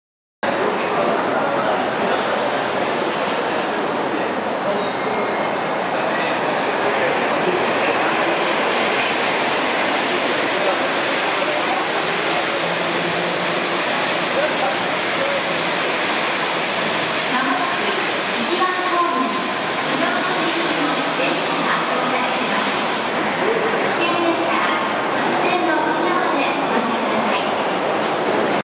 【フルHD】大阪高速鉄道大阪モノレール線1000系 千里中央(15)駅発車 1(京急号)
21編成(京急号)。
当駅止まりで引き上げ線へ回送されます。